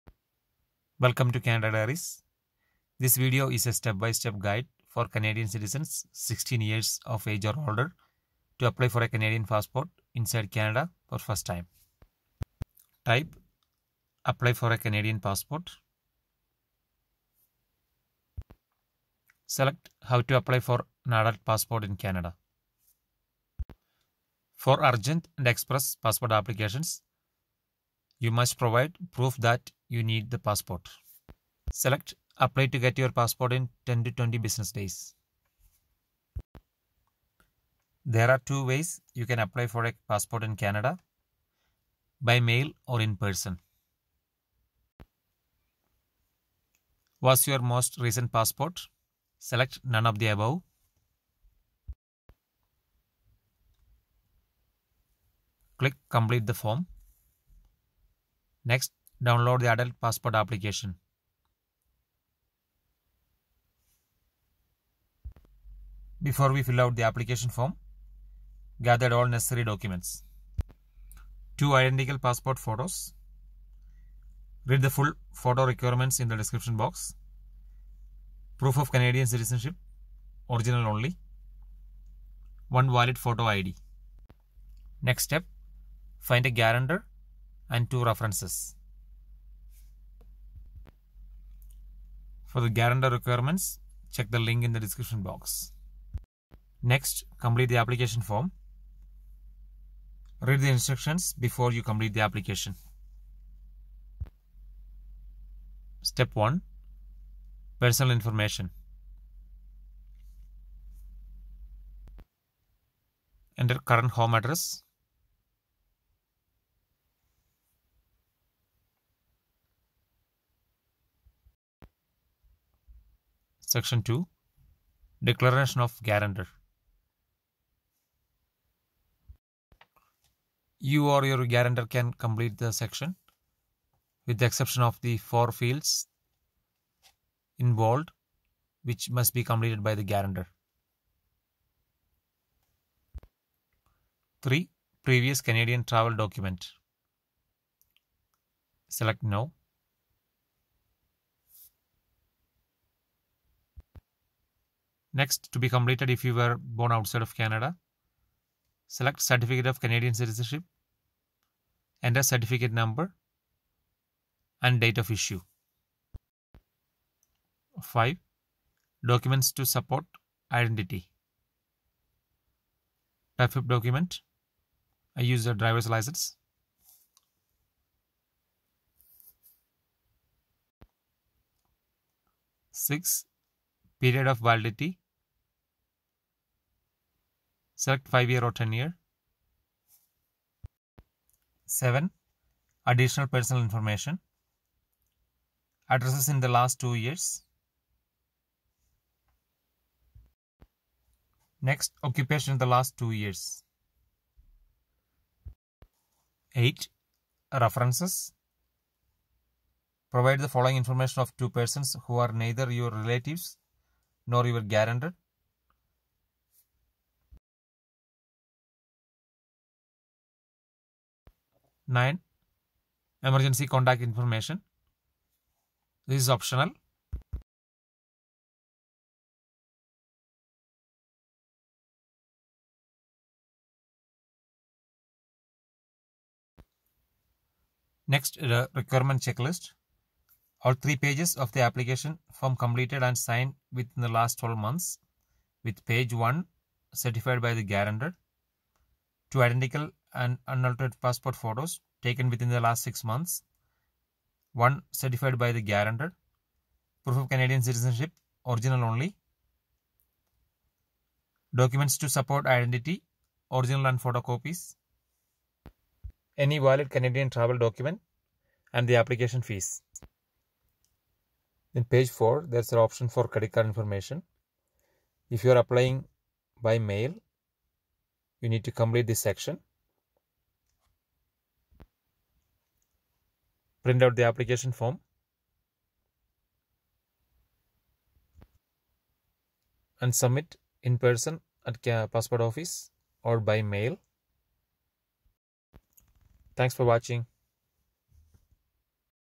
Apply for an adult Canadian passport application (New citizen) step by step guide
This video is a step-by-step guide for Canadian citizen 16 years of age or older to apply for a new Canadian passport from inside Canada.
There are two ways you can apply for a passport from Canada: by mail or in person.

Canadian passport photo requirements

Guarantor information